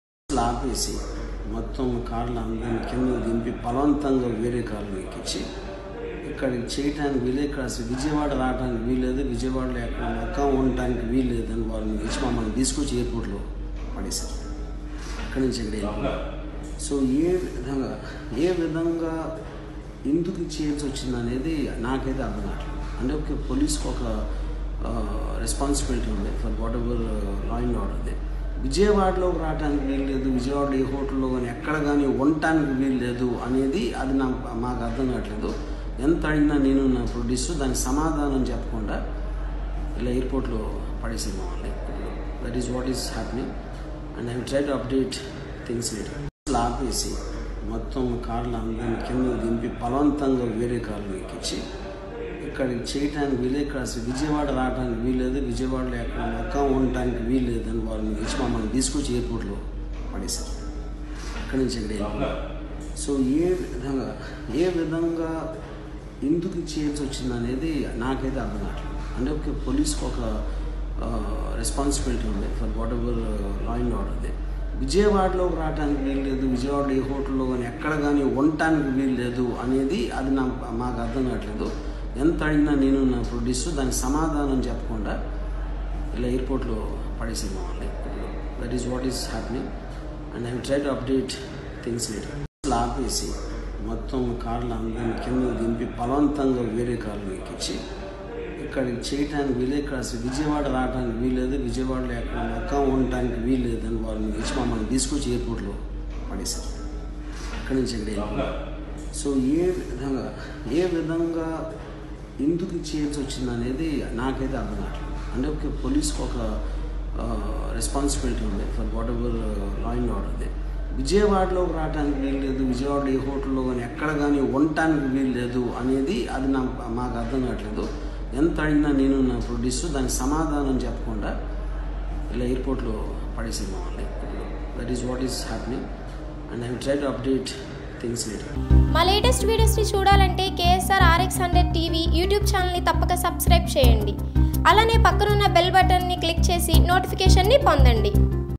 RGV About AP Police | Ram Gopal Varma Arrested At Vijayawada | KSR RX 100 TV
పోలీసుల వైఖరి నాకు అర్ధం కావడం లేదు | రామ్ గోపాల్ వర్మ  | KSR RX 100 TV